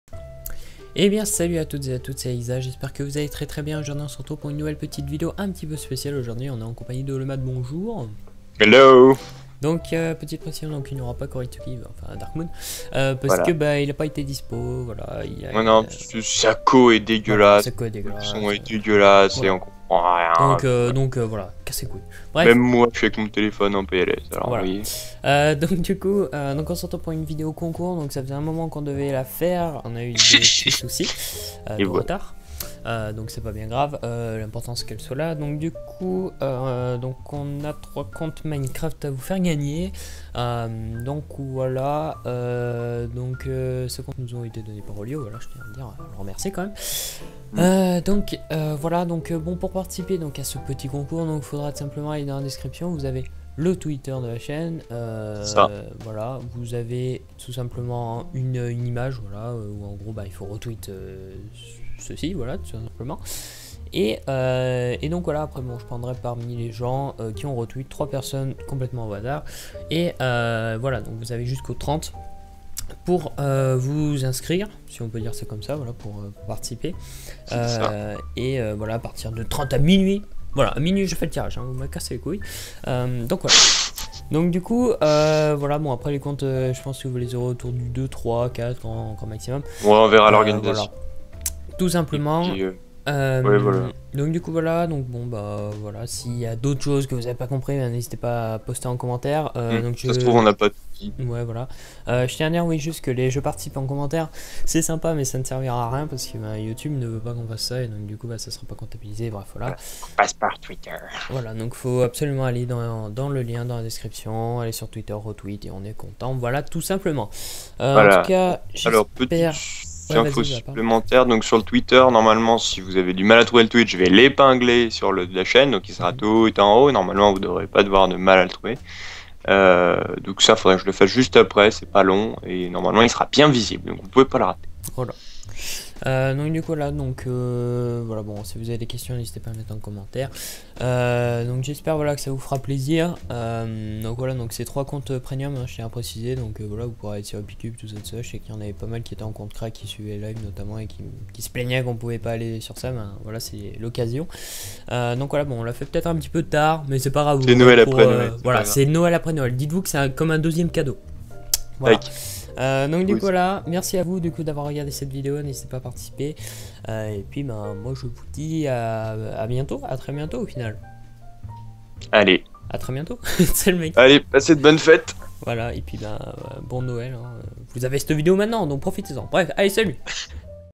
CONCOURS [TERMINÉ] : GAGNE UN DES TROIS COMPTES MINECRAFT PREMIUM
Yo bande de Roger c'est Exa, Matt ( et Kuraitsuki, dans nos coeurs !) et on se retrouve pour un petit concours à l'occasion de NOËL. Tente de gagner un des trois comptes minecraft avant le 30 minuit ! Pour ceci il faut RT le Tweet prévu à cette effet
Si jamais laisse un com ici ( Ceux qui n'ont pas TWITTER
Bonne vidéo !